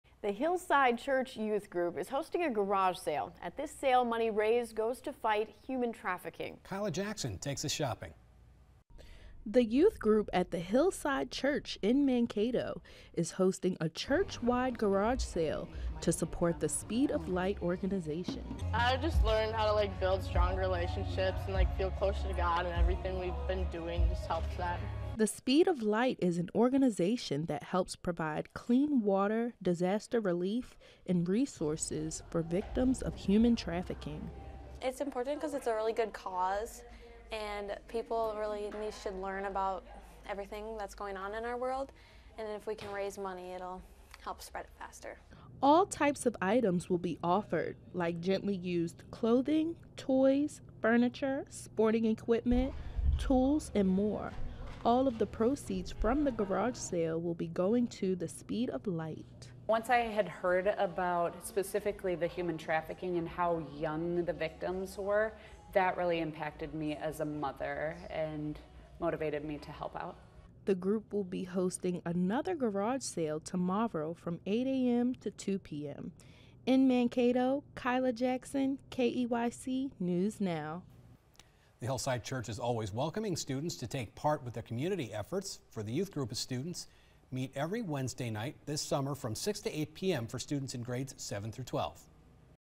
Hillside Church to host garage sale to fight human trafficking
The youth group at the Hillside Church in Mankato is hosting a church wide garage sale to support the Speed of Light organization.